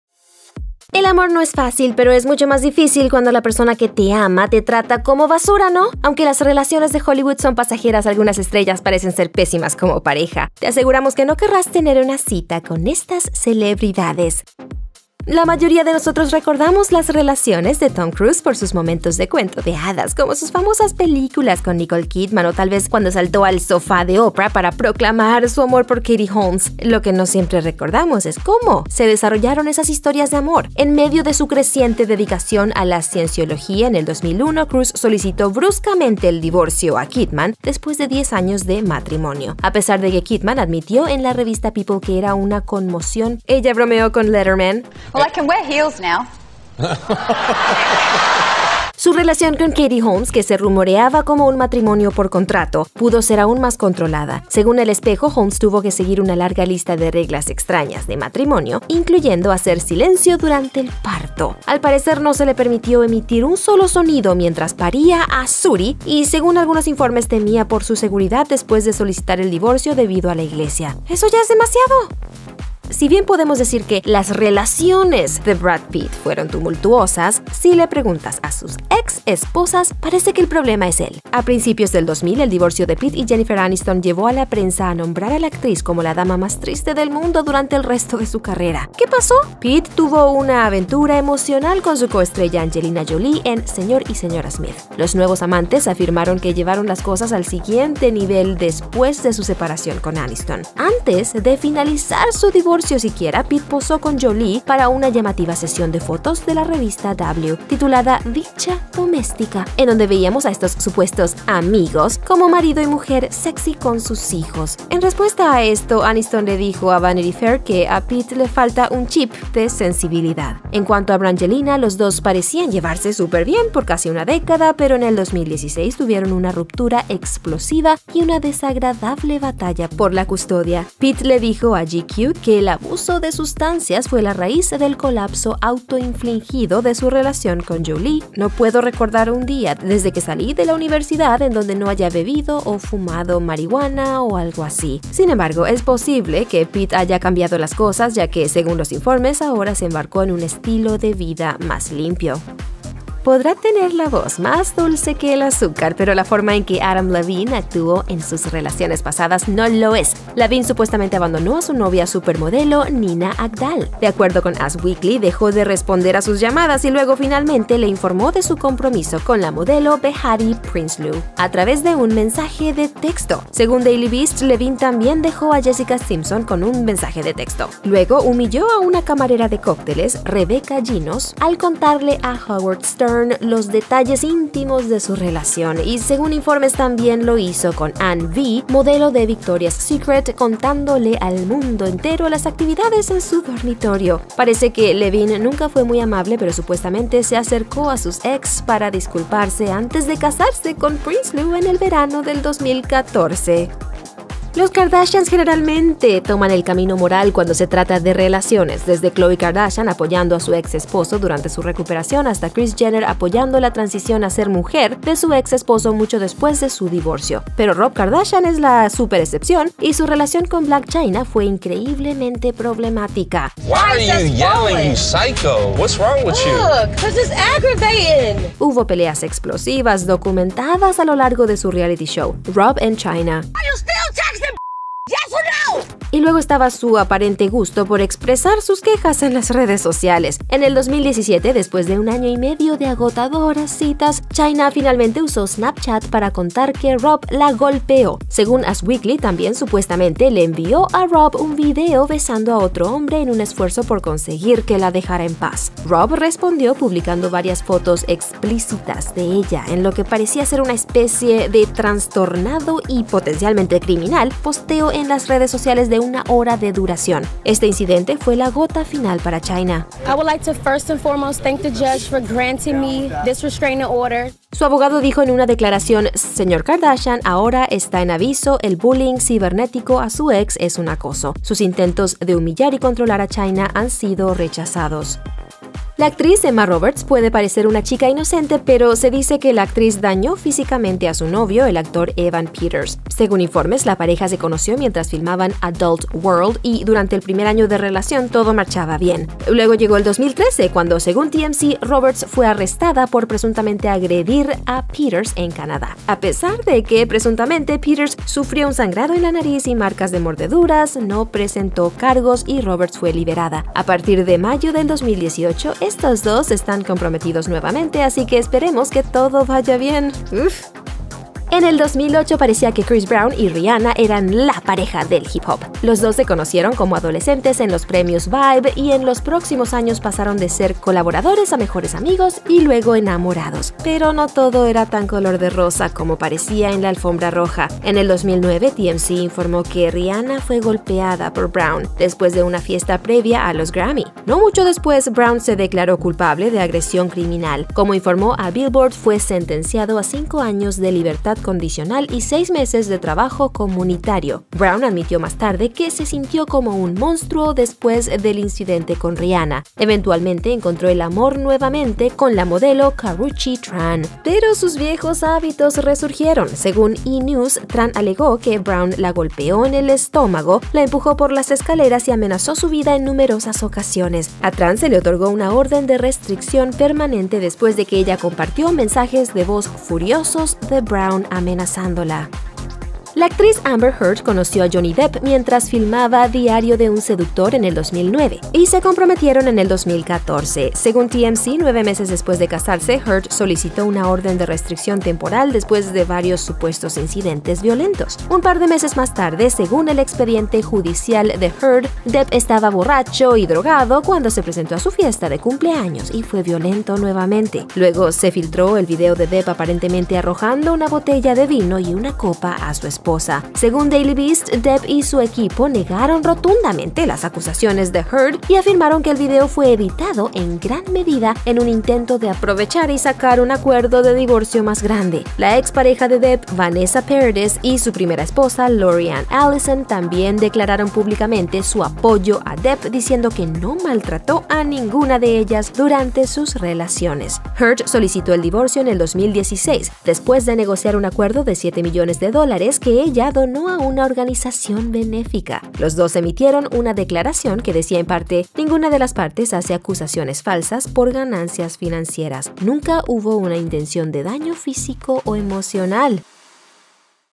Celebridades Que Trataron Como Basura A Sus Parejas
El amor no es fácil, pero es mucho más difícil cuando “la persona que te ama” te trata como basura, ¿no? Aunque las relaciones de Hollywood son pasajeras, algunas estrellas parecen ser pésimas como pareja. Te aseguramos que no querrás tener una cita con estas celebridades.

La mayoría de nosotros recordamos las relaciones de Tom Cruise por sus momentos de cuento de hadas, como sus famosas películas junto a Nicole Kidman, o tal vez cuando saltó al sofá de Oprah para proclamar su amor por Katie Holmes. Lo que no siempre recordamos es cómo se desarrollaron esas historias de amor.

En medio de su creciente dedicación a la Cienciología en el 2001, Cruise solicitó bruscamente el divorcio a Kidman después de 10 años de matrimonio.

Su relación con Katie Holmes, que se rumoreaba como un matrimonio por contrato, pudo ser aún más controlada. Según El Espejo, Holmes tuvo que seguir una larga lista de reglas extrañas de matrimonio, incluyendo hacer silencio durante el parto. Al parecer, no se le permitió emitir un solo sonido mientras paría a Suri, y según algunos informes, temía por su seguridad después de solicitar el divorcio debido a la iglesia. ¡Esto ya es demasiado!

Mira este video para saber más acerca de celebridades que trataron como basura a sus parejas.

Tom Cruise | 0:14
Brad Pitt | 1:12
Adam Levine | 2:33
Rob Kardashian | 3:25
Emma Roberts | 4:46
Chris Brown | 5:21
Johnny Depp | 6:23